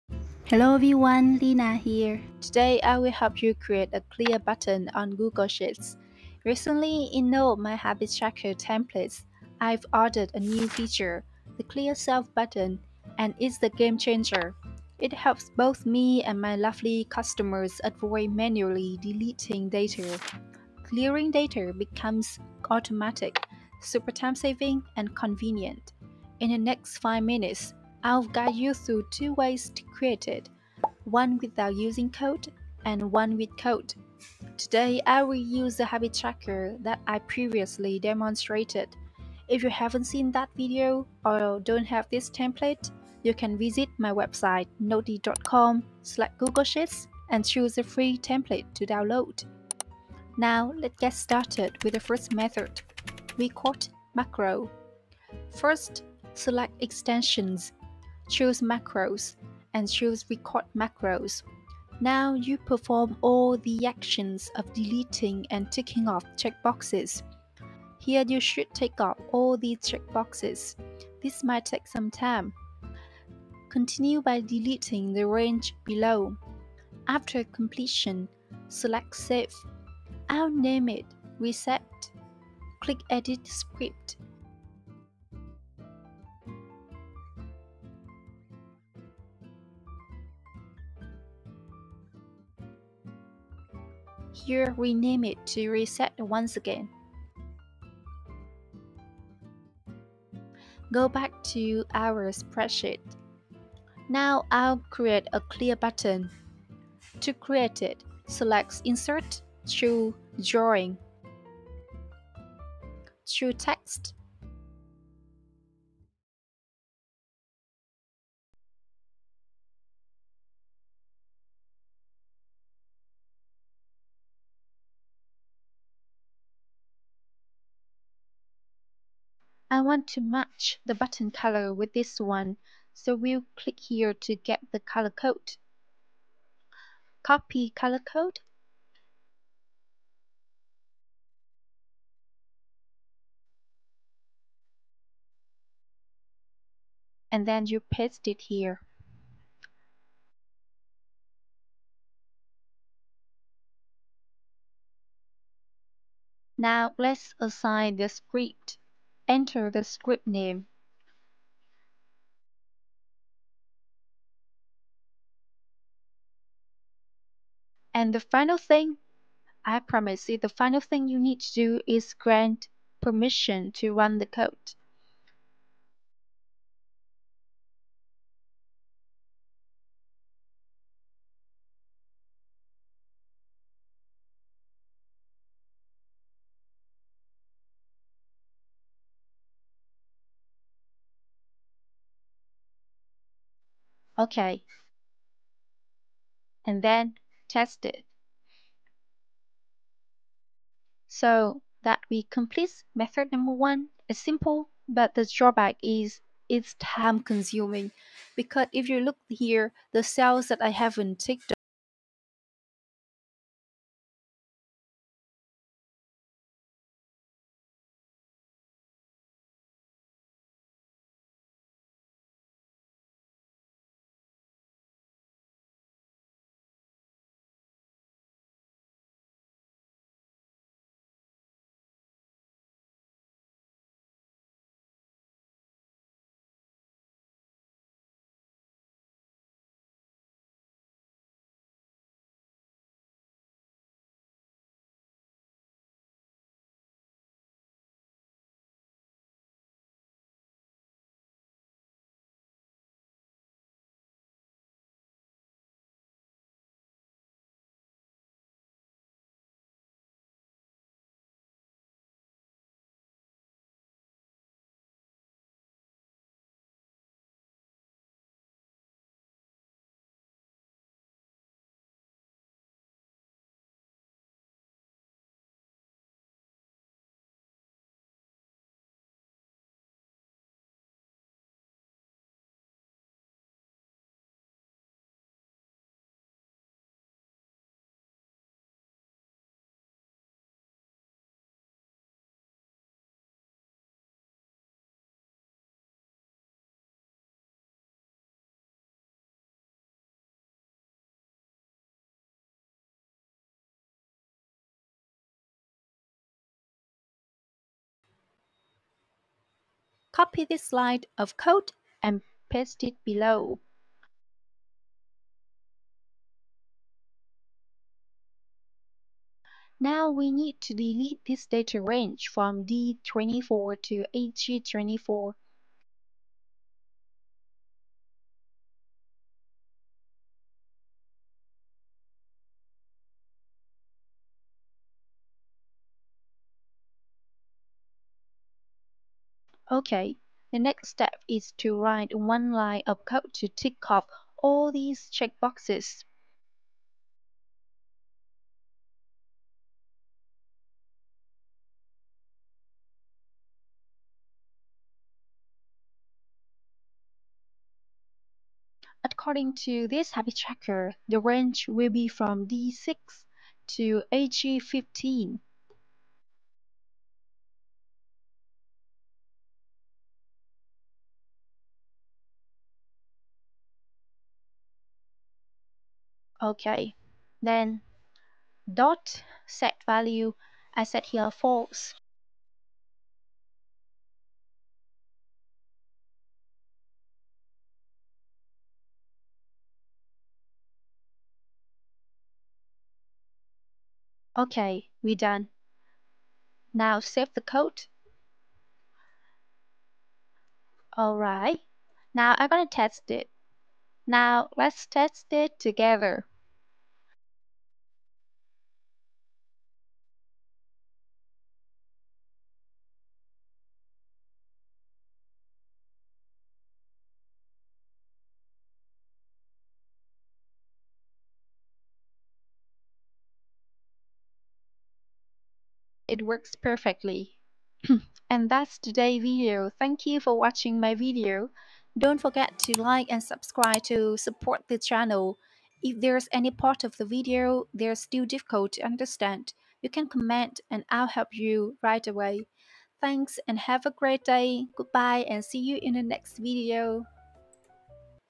How To Apply A Button To Clear Specific Cells In Google Sheets in 2024
In the next 5 minutes, I'll guide you through 2 ways to create it. One without using code and one with code.
►Video section
0:00 : Intro
0:50: Way 1 - Macro
3:29: Way 2 - Code
google sheets, excel, code, appscripts, macros